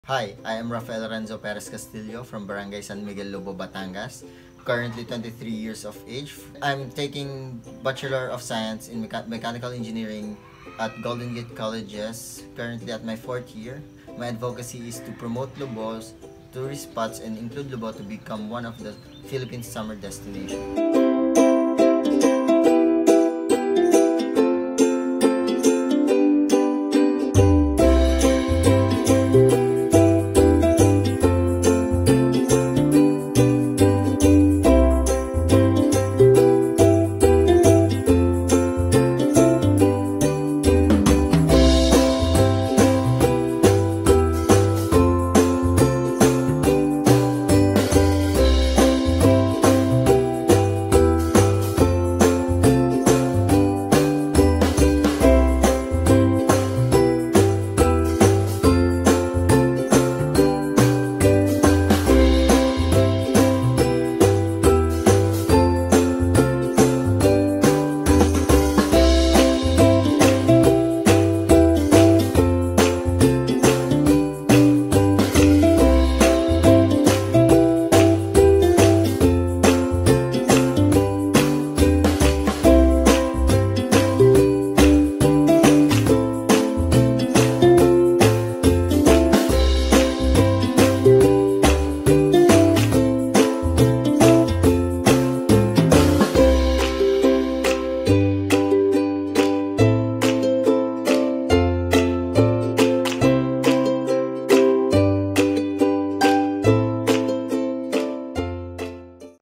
Mister Lobo 2019 candidate No. 16 - Raphael Renzo Castillo
Advocacy:

To help Lobo's tourism in promoting tourist spots and to become one of the Philippine's best summer destination